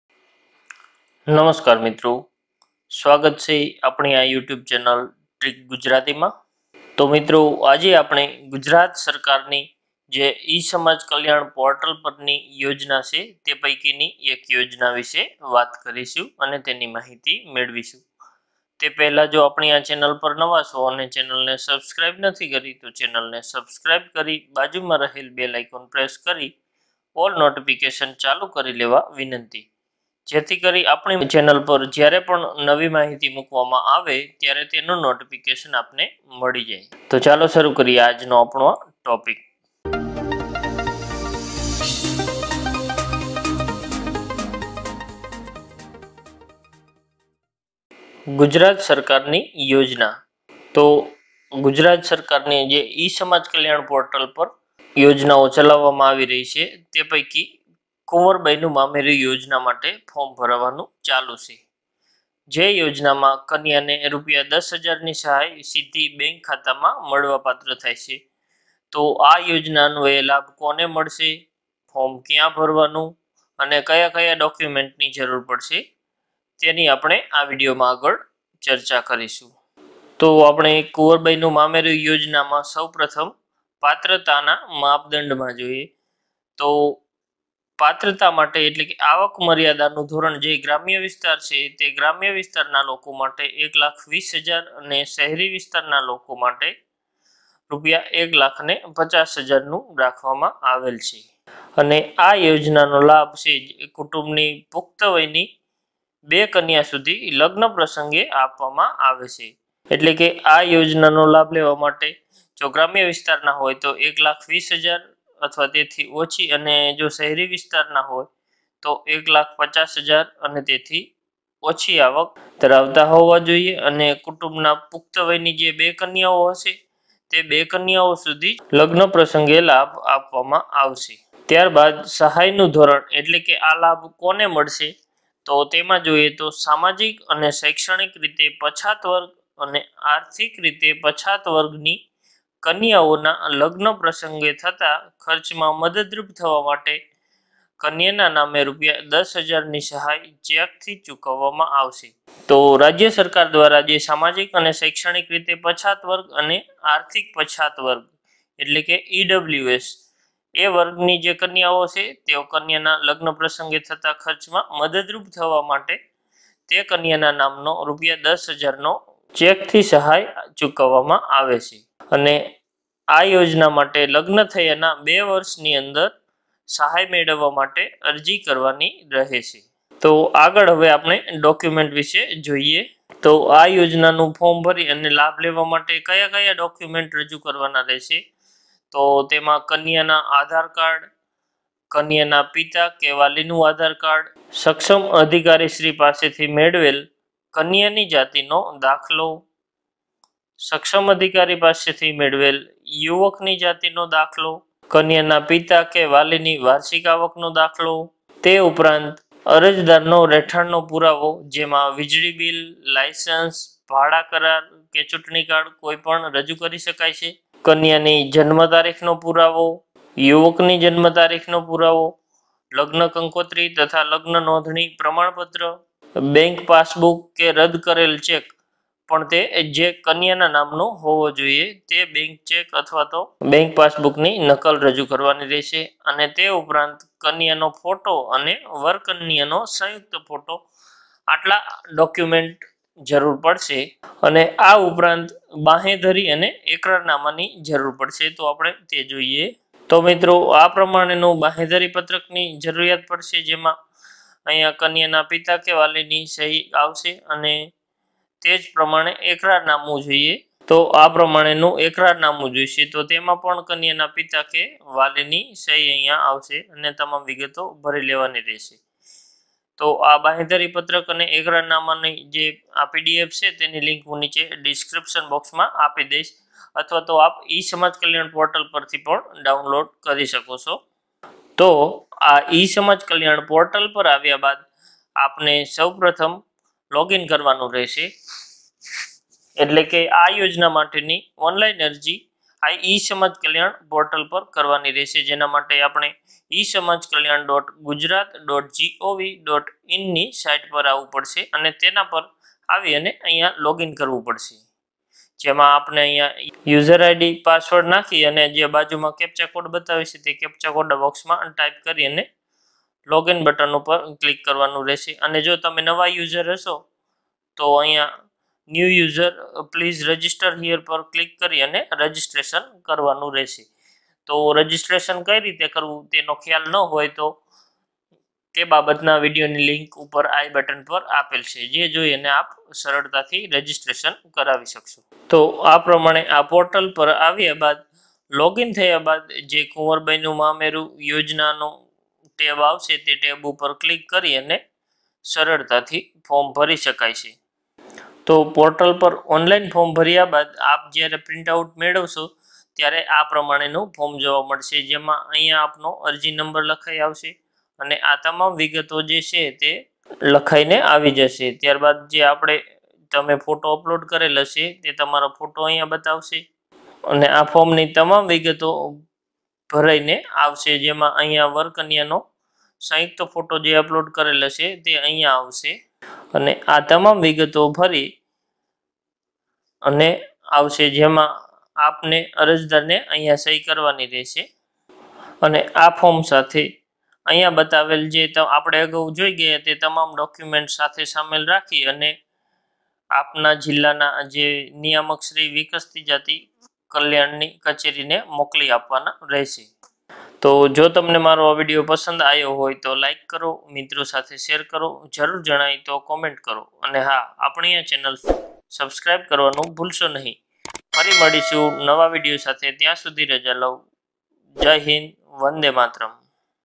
કુંવરબાઇનું મામેરૂ યોજના || Kunvarbai nu Mameru Yojana || e-samajkalyan Yojana || By Trick Gujarati
મિત્રો આ વિડયોમાં ગુજરાત સરકારની કુંવરબાઈનું મામેેરું યોજના ની માહિતી આપવામાં આવી છે.